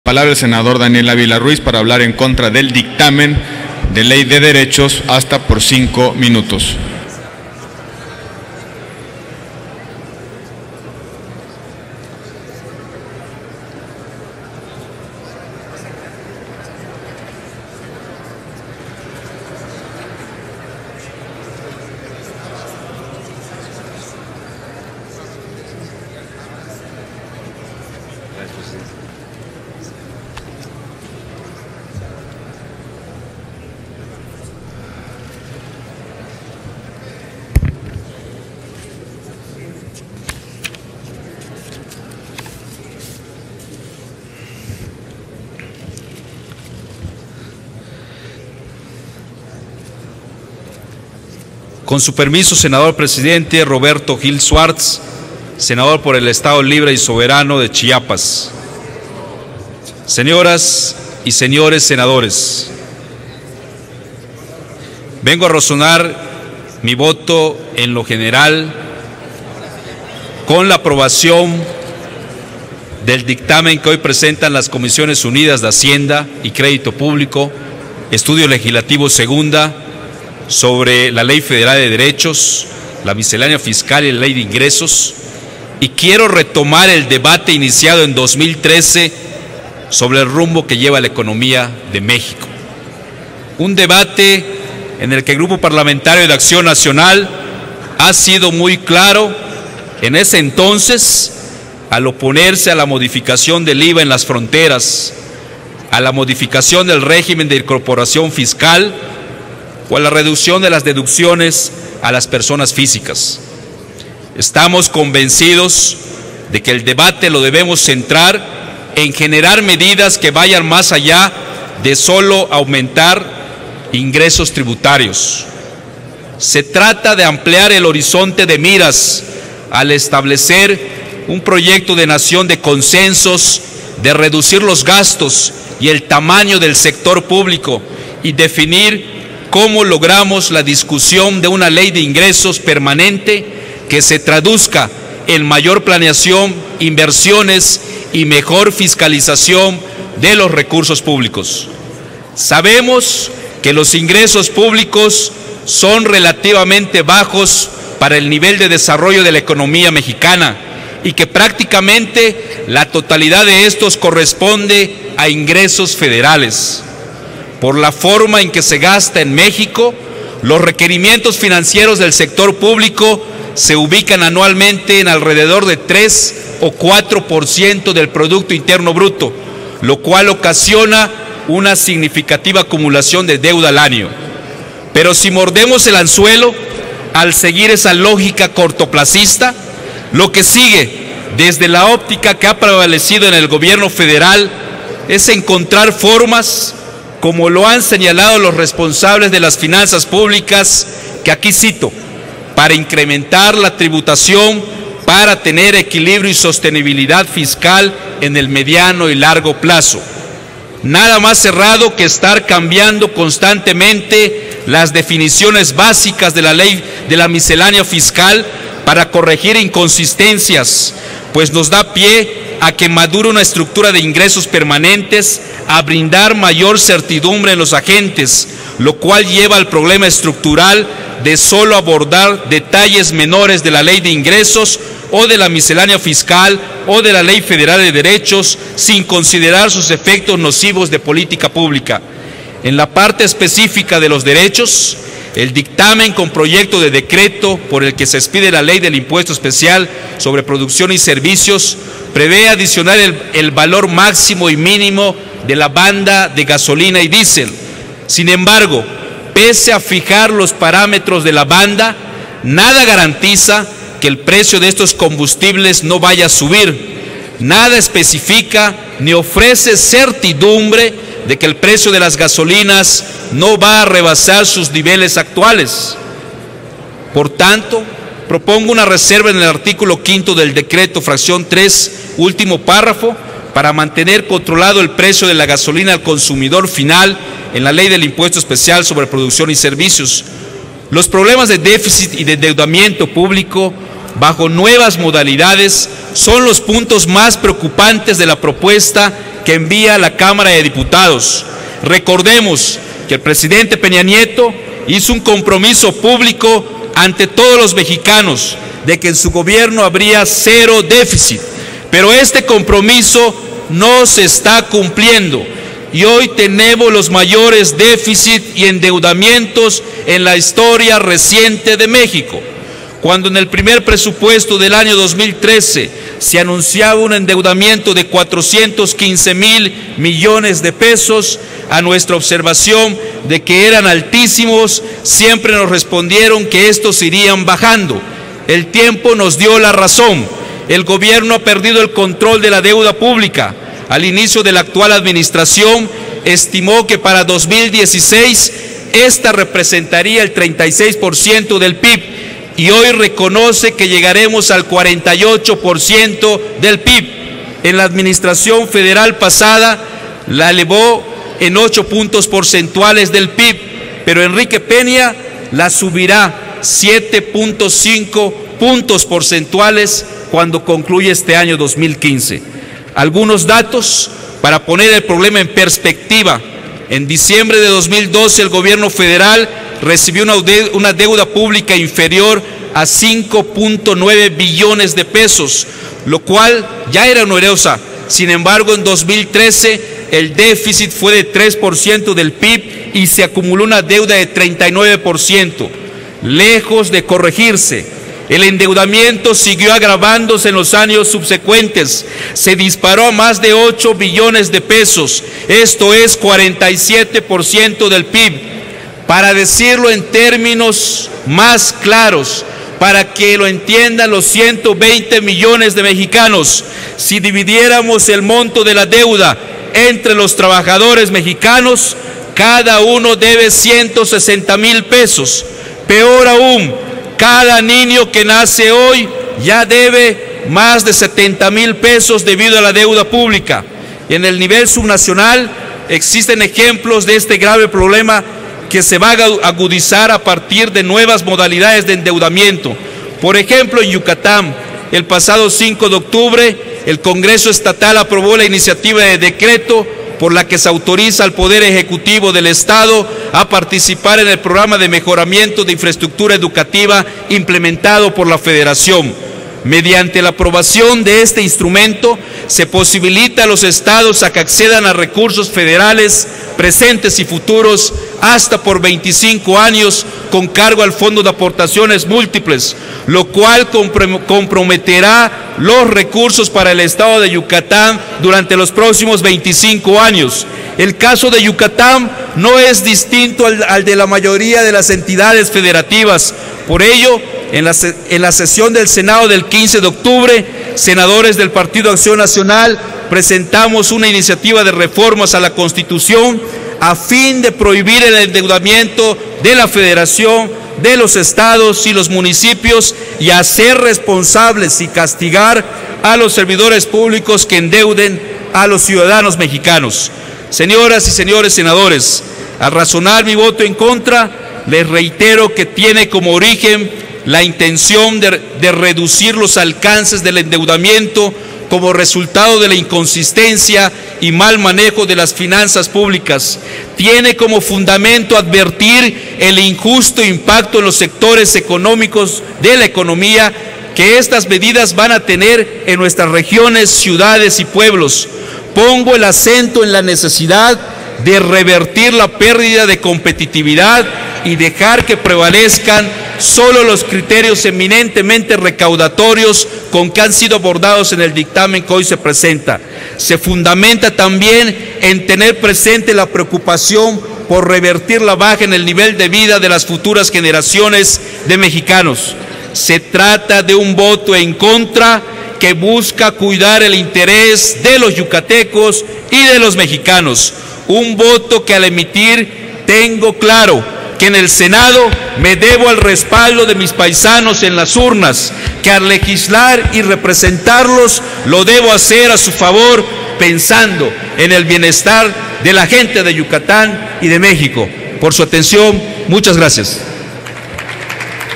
Sen. Daniel Ávila enfatiza que se deben reducir los alcances del endeudamiento
Sen. Daniel Ávila en contra del dictamen que reforma la Ley Federal de Derechos. El senador destacó que su voto en contra se debe a que se deben reducir los alcances del endeudamiento.